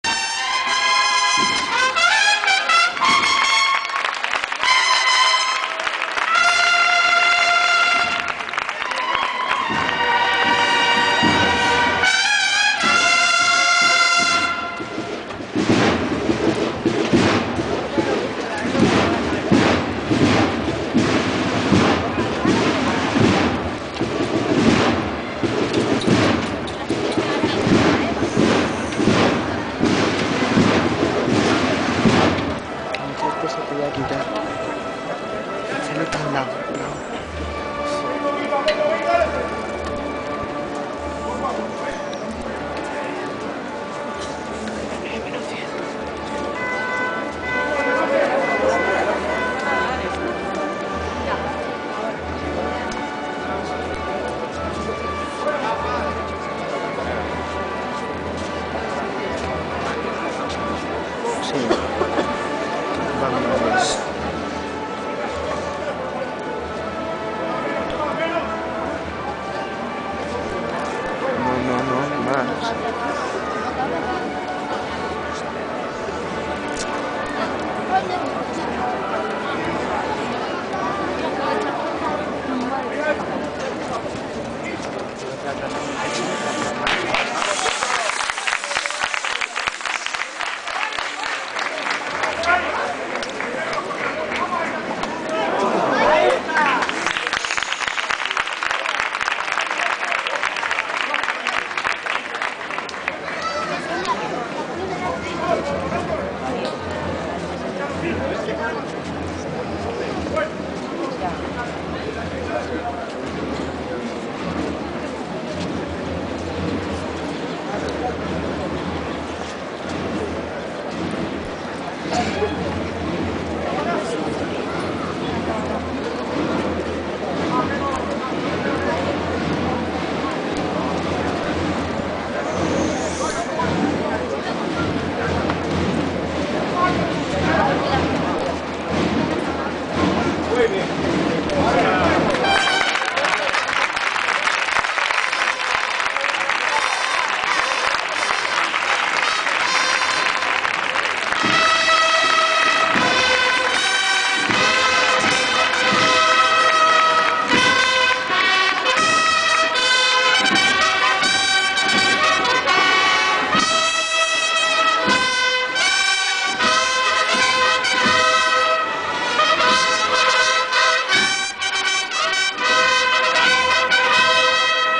Entrada Virgen Amargura.Sábado Santo de 2007 en Ciudad Real
Entrada del paso de María Santísima de la Amargura en la iglesia de San pedro la tarde de sábado santo de 2007. Ciudad Real